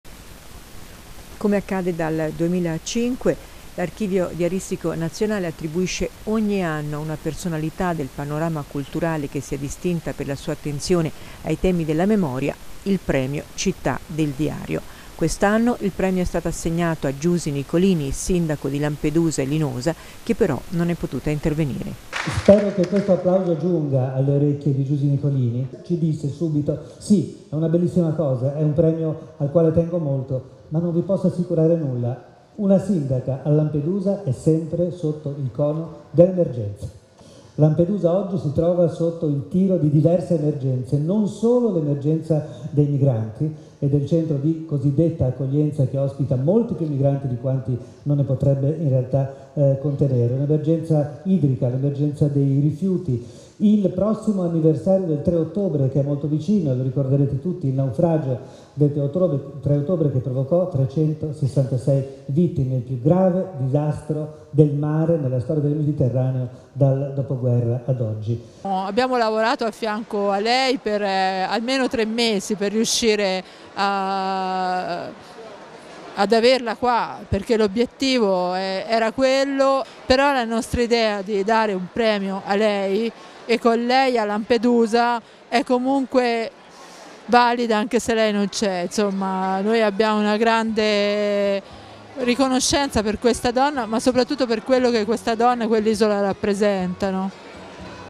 Nel Tg il Premio Città del Diario 2016 a Giusi Nicolini
Il Premio Città del Diario 2016 è stato assegnato a Giusi Nicolini, Sindaco di Lampedusa, che per le emergenze in corso, non è potuta intervenire ce ne parla Guido  a Giusi NicoliniUn servizio in onda sul canale digitale terrestre di Teletruria 2 -Tvs  a cura dell' a.c. Centodue SubbianoTv realizzato per la promozione del territorio da Stefanella Baglioni e Fabrizio Radicchi.